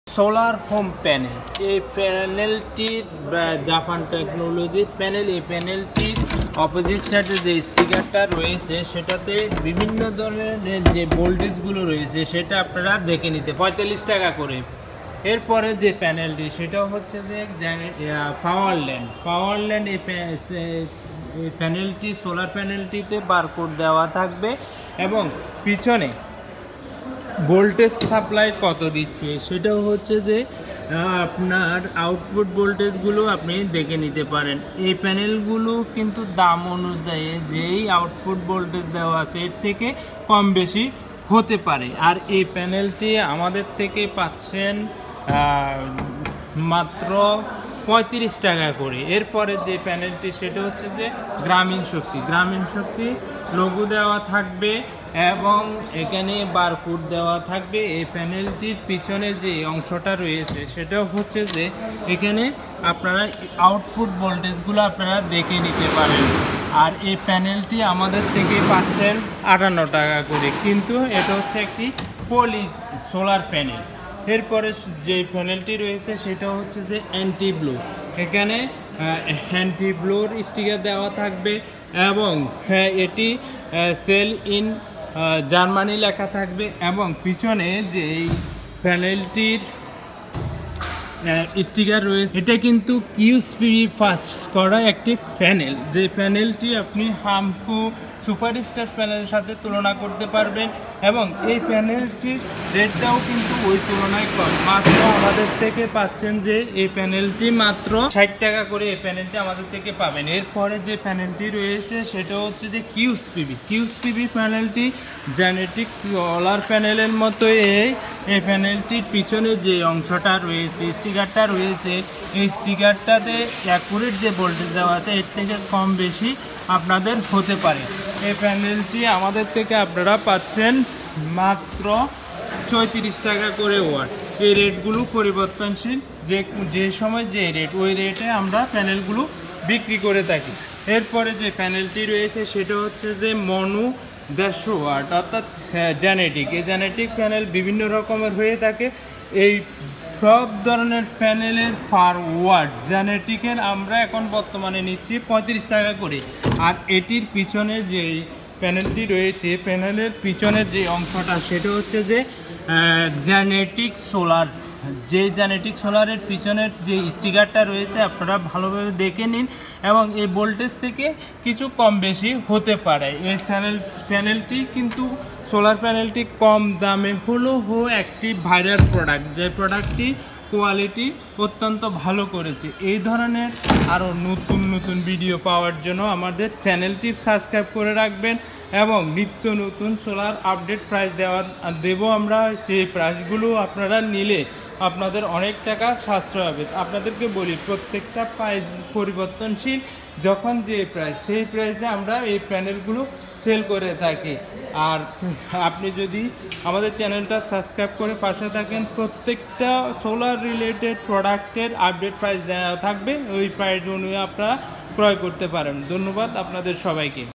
SOLAR PANEL PRICE IN BANGLADESH
Video Credit :
The prices of solar panels in Bangladesh may vary depending on the quality and advanced technology
Eg 1.Solar Home Rs.40
  2. Anti Blue 60 Rs
  3.Genetic 35 Taka
4. Grameen Shakti 58 rupees
5. QSPB makes Rs.35
The rate is variable We provide updated videos as soon as rates change We sell solar panels at very low prices in Bangladesh
to all of you